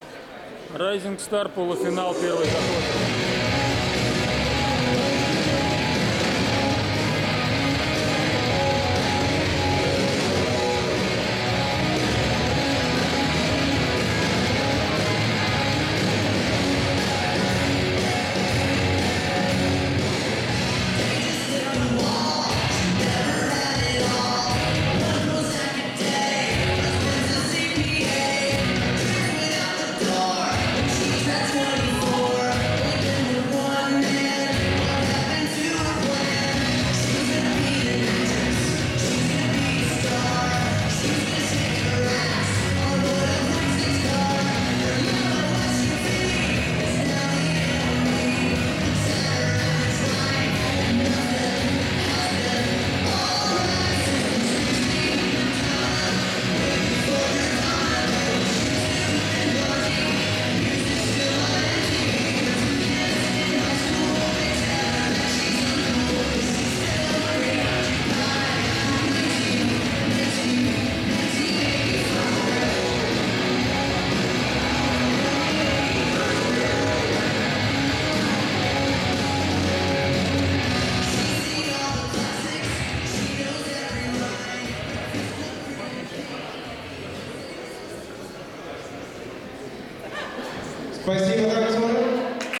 17.9.16 Открытие сезона 1/2 DnD Rising Star заход 1 смена 1
Открытие сезона по хастлу и дискофоксу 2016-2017 года
17 сентября2016 г.
Место проведения: ТДЦ Буревестник
Организаторы проведения: Ассоциация Спортивного Хастла
Непосредственные организаторы: Хастл-центр, Елена Кетуровская-Козлова, Андрей Григорьев и Ко
видеооператор: Игорь Скобелев